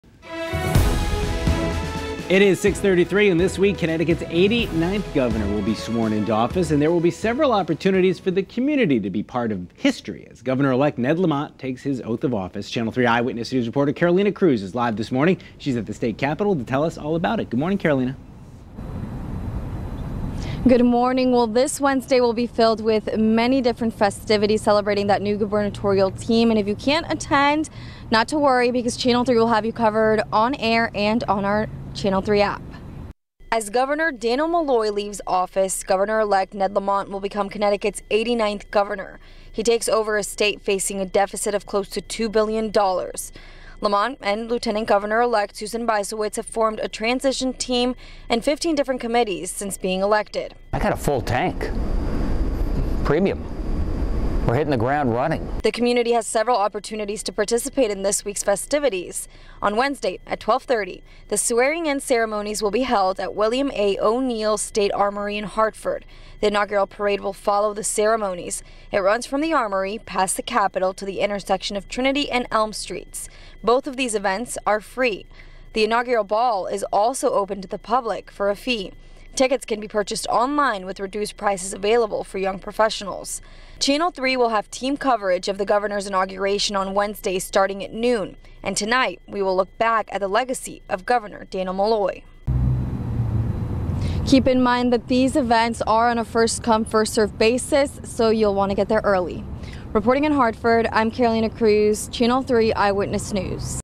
VIDEO: CT's 89th gov. to be sworn in on Wednesday
Gov.-elect Ned Lamont will be sworn in on Wednesday.